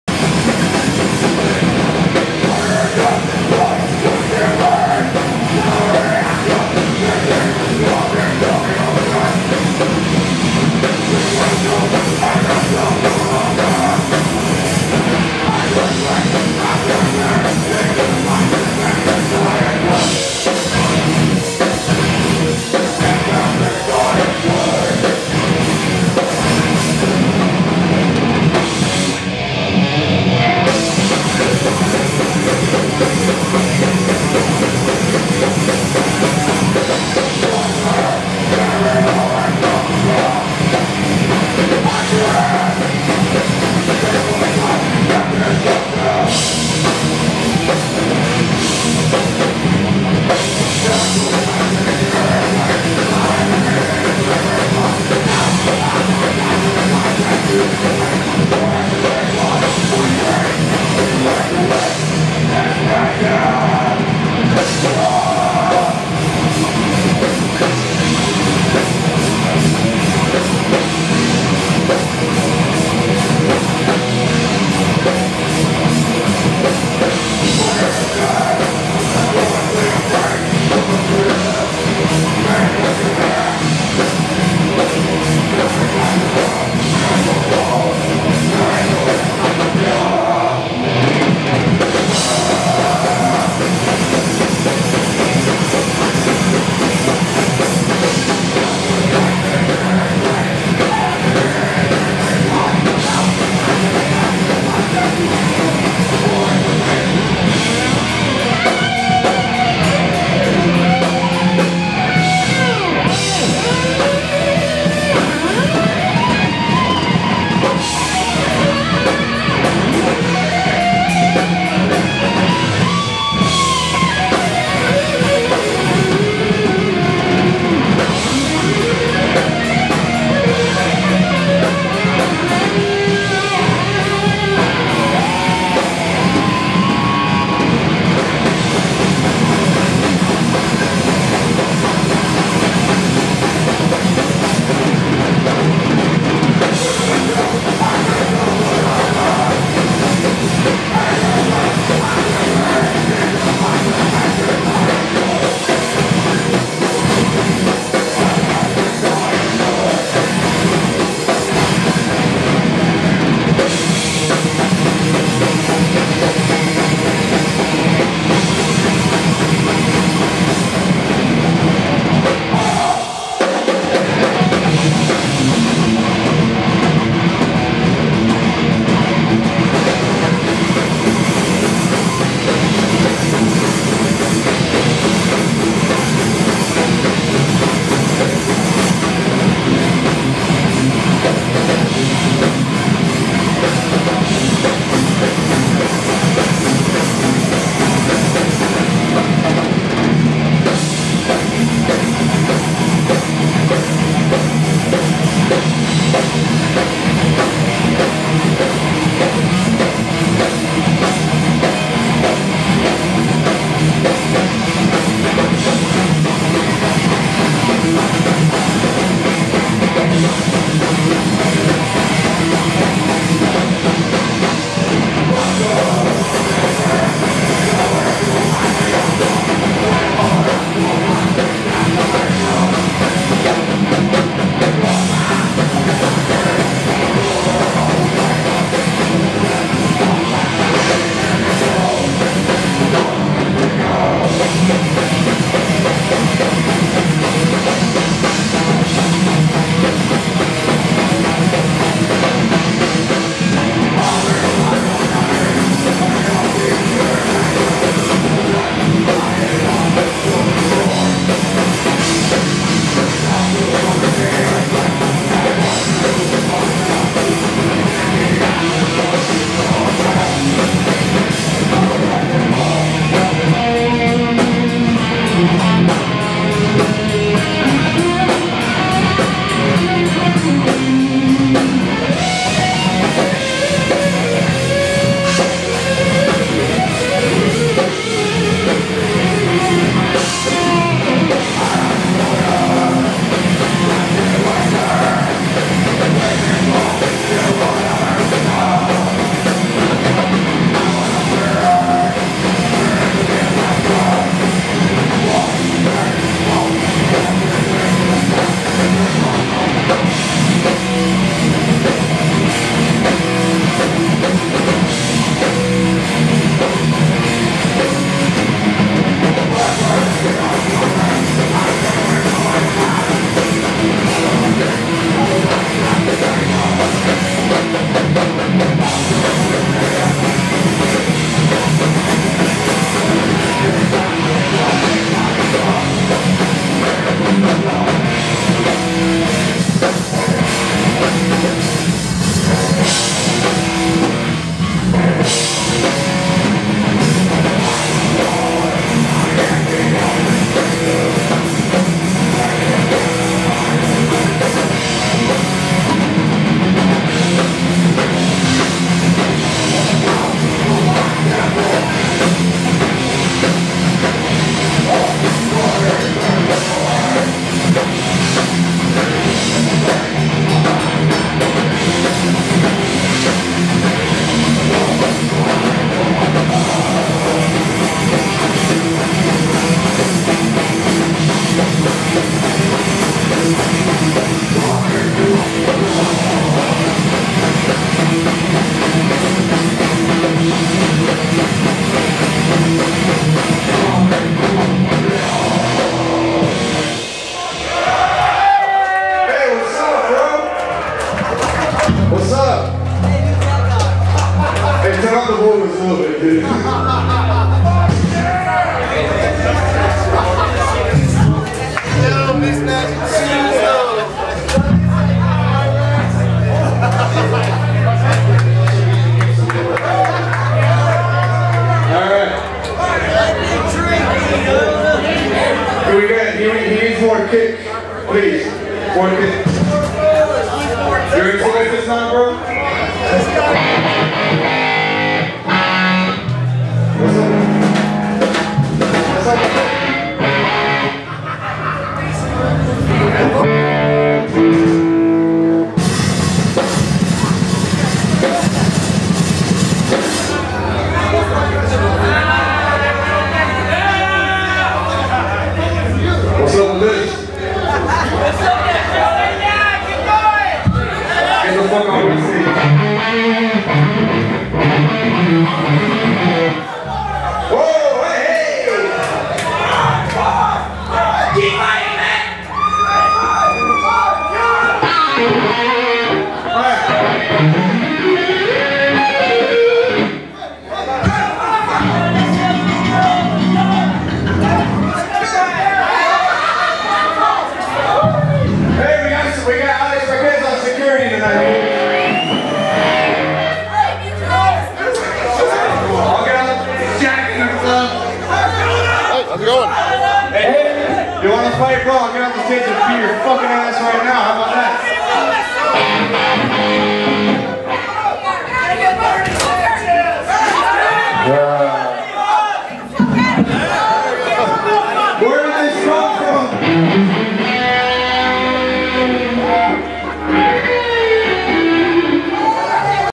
Solstice @ Churchill's 3/21/15
Come join us as we show our support for this blackened death metal band from San Antonio, Texas: HOD, will be making their first appearance in Miami, Florida. Also appearing as our co headline band will be, Kult ov azazel, South Florida black metal legends will be sharing a stage with them.
Doom death metal band from Orlando, Druid Lord will also making their first South Florida appearance.

Also performing is Solstice, Gnosis, and Koroida.

Come out to this blackened, thrash, doom, death metal night. Show your support to the scene at Churchill's Pub.

18+

Door cover: $15

Gnosis ( Featuring members from Devastator, Nekromaniak, and Sinistre Division) : 9:30pm – 10pm

First band at 8:30pm

Doors at 8pm